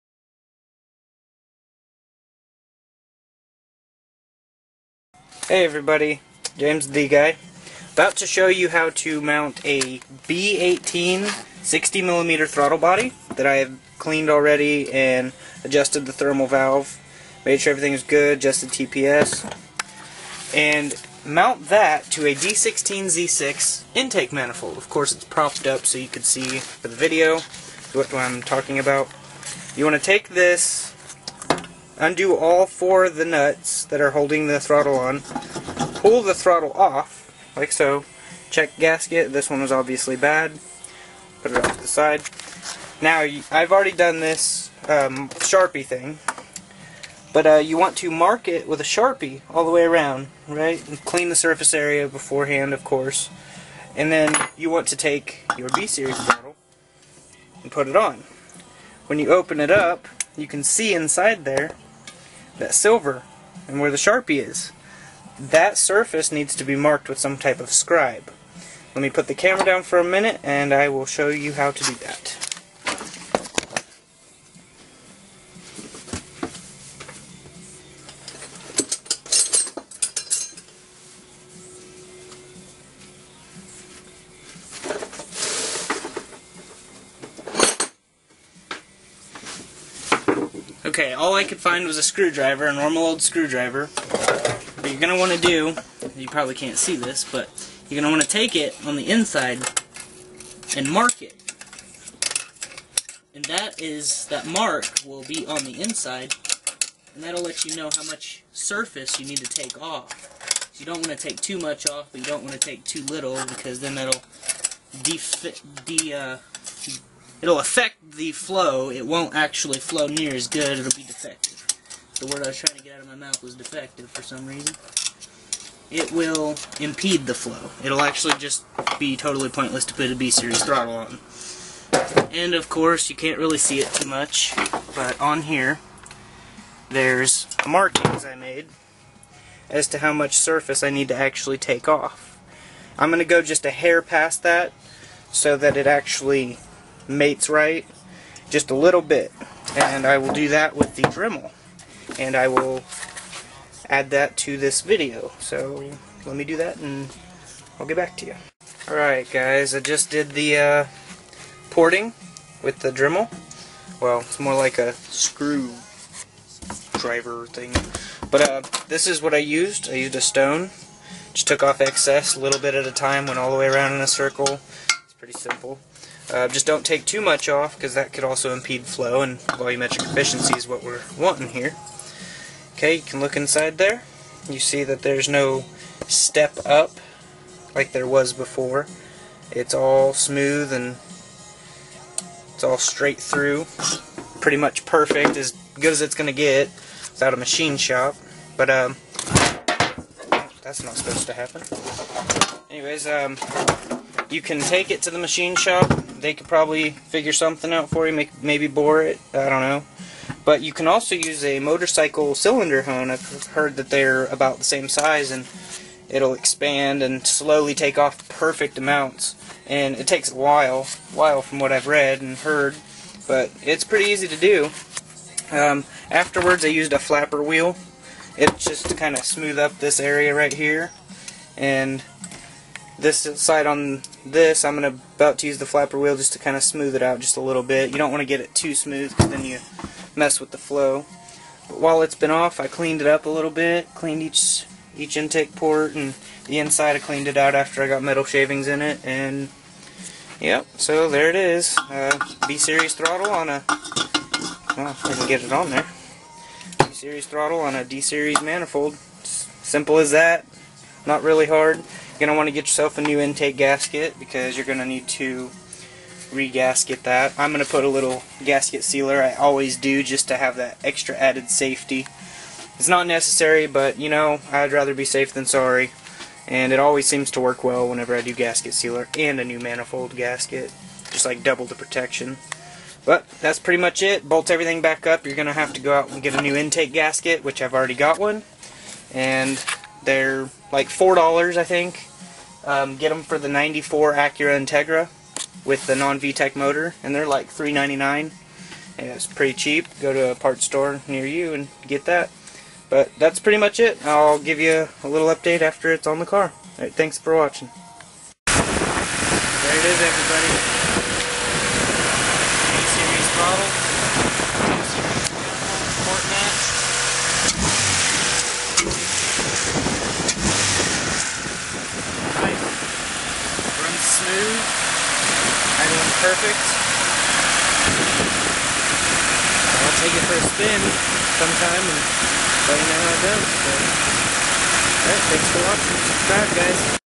How to port match
How to port match the B-series throttle to a D16z6 intake manifold.